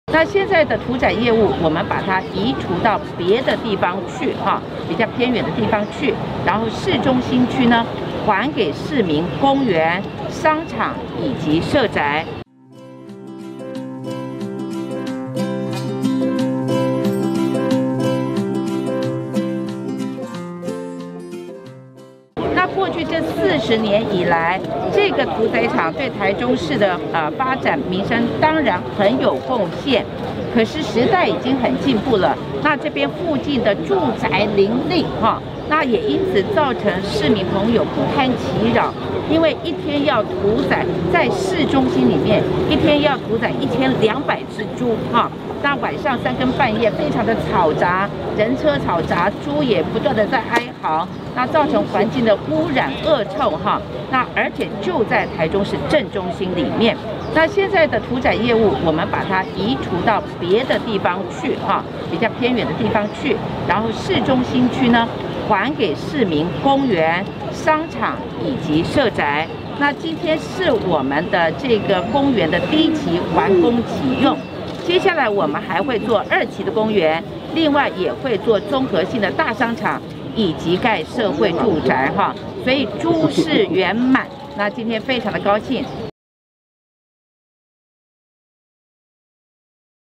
台中居民高興開窗 屠宰場變豬事圓滿公園
台中市肉品市場轉型不再屠宰後，市府改建為公園、商場和社會住宅，今天公園第一期工程完工，市長盧秀燕和地方人士共同揭幕啟用。